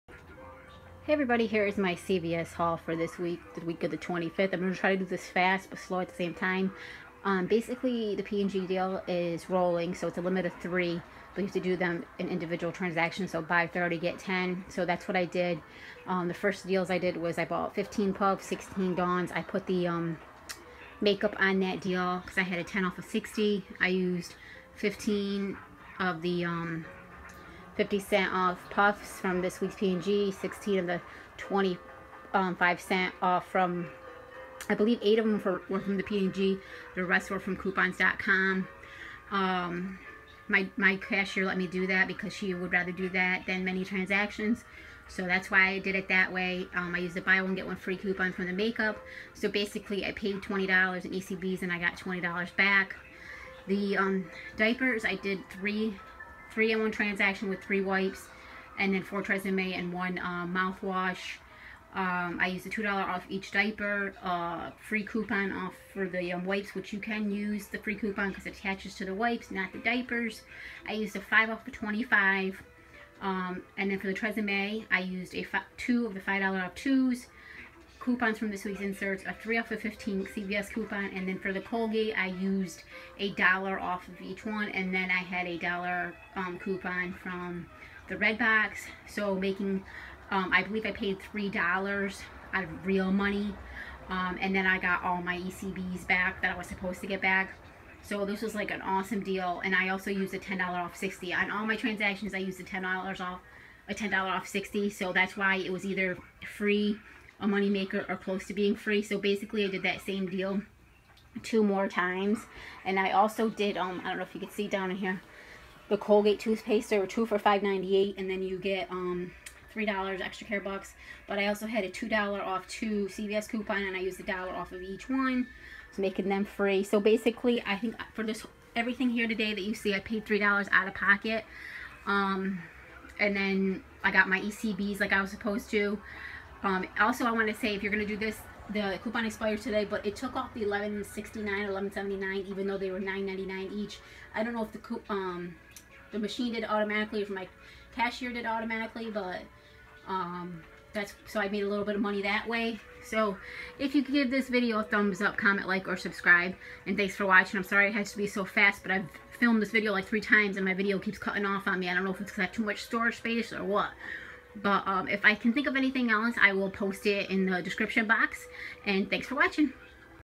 Cvs Couponing Haul 09/25/16
I did (4) transactions and i used (4) $10 off $60 one for each transactions .. i also used .50 pg coupons for puffs .25 dawn pg and coupons.com.. revlon used b1g1 coupons expires today.. also used a$3/15 makeup coupon from cvs redbox and used $3 revlon coupon from redbox.. pampers used $2 off each one and used the free wipes coupons for each one.. used (3) $5/$25 cvs coupons one per transaction ... used (4) $5/2 tresemme coupons $3/15 cvs shampoo coupon.. used (4) $1/1 toothpaste coupons and $2/2 cvs coupon for toothpaste and lastly $1/1 man coupon for colgate mouthwash and used $1/1 cvs coupon and (2) $2/1 cvs coupons for mouthwash one per transactions.. thanks again for watching!